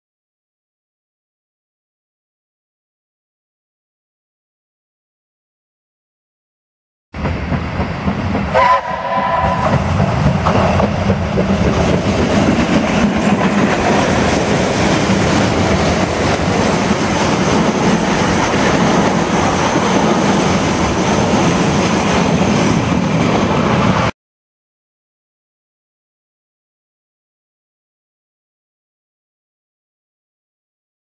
Steam train emerges from tunnel
A cool old steam train emerging from a tunnel on the Kaikoura coast, South island, New Zealand.